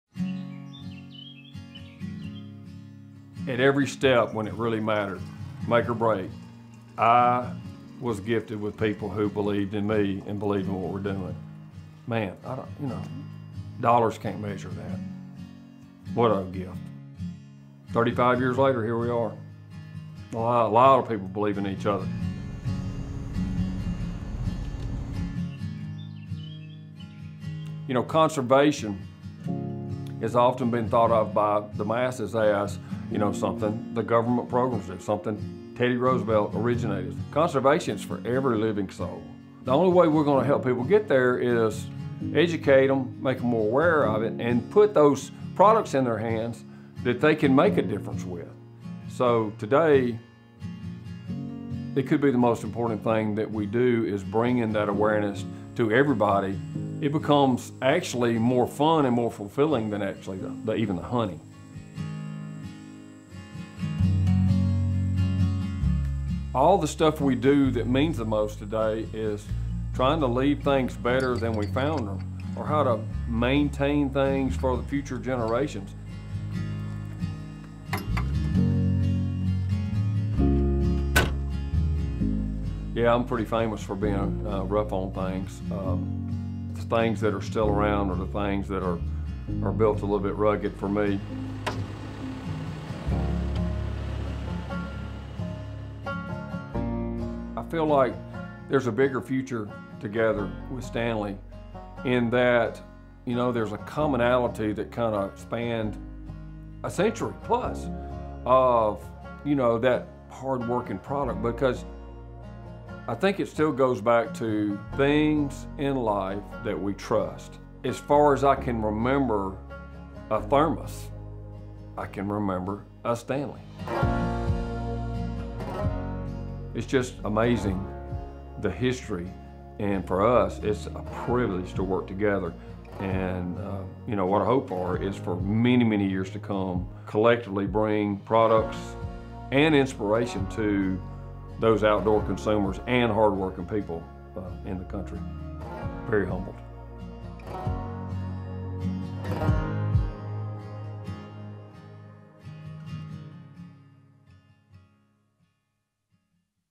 Stanley x Mossy Oak: Uniting Two Legends
Mossy Oak and Stanley come together to create a memorable collection rooted in heritage and love for the outdoors. Featuring Mossy Oak's signature Bottomland and Country DNA patterns and Stanley's legendary bottles, mugs, and flasks, these products were built for hunt, fish, work and wherever else you go.